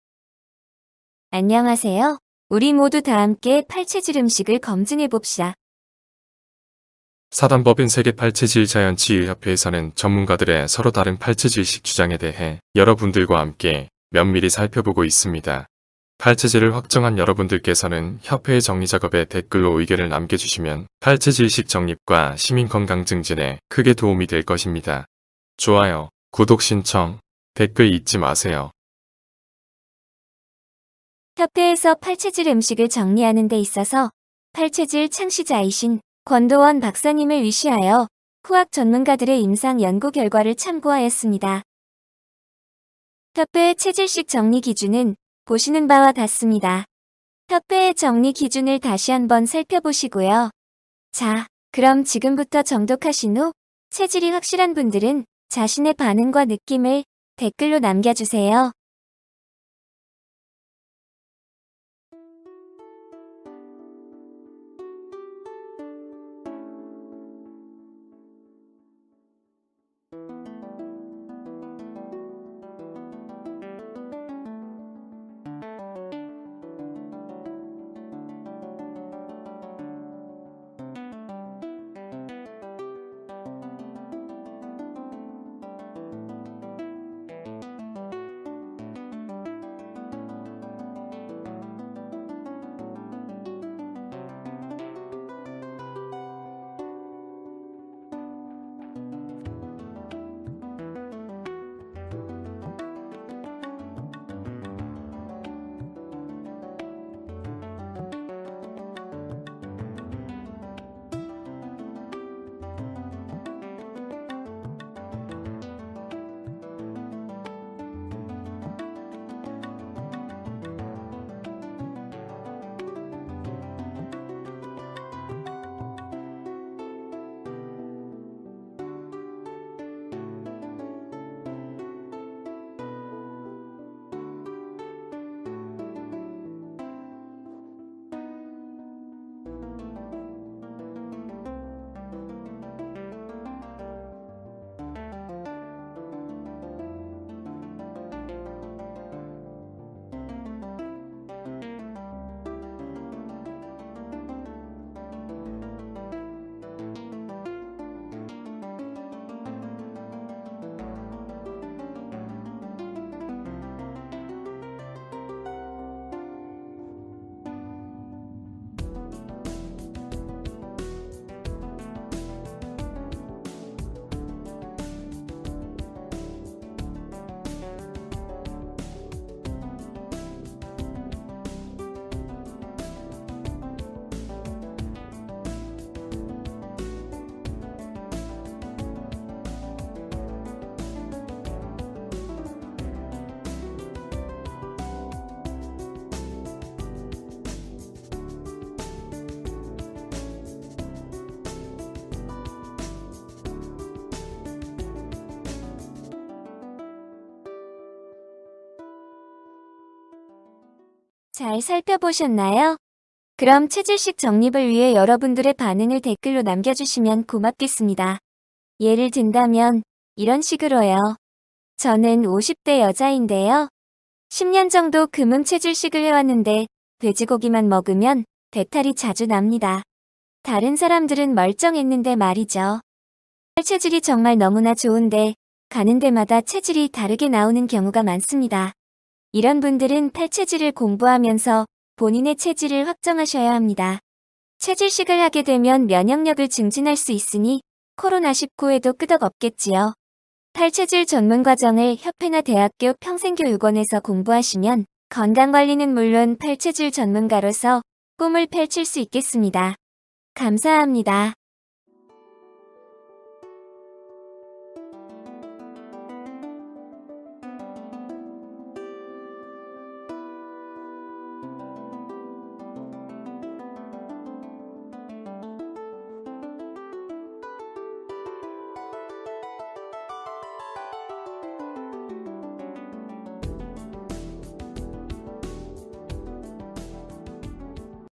8체질식 검증(179) : 치콘, 케일, 콜라비
우리 모두 다 함께 체질식을 검증해봅시다. ​ ​​ ​​ ​ ​ ​ ​ ​ ​

WECNA(8체질협회) 정리 :
1.치콘 : 목.수체질은 해롭고 금.토체질은 이롭다는데, 임상 결집이 필요하다.
2.케일 : 목.수체질은 해롭고 금.토체질은 이로운 것으로 정리되는데, 임상 결집이 필요하다.
3.콜라비 : 금.토음.수양체질은 해롭고 목.토양.수음체질은 이롭다는데, 임상 결집이 필요하다. ​

여러분의 소중한 댓글이 8체질의학의 정립을 앞당깁니다~ ​ ​ ​​

​ * 자막 음원 출처 : 클로버 더빙 ​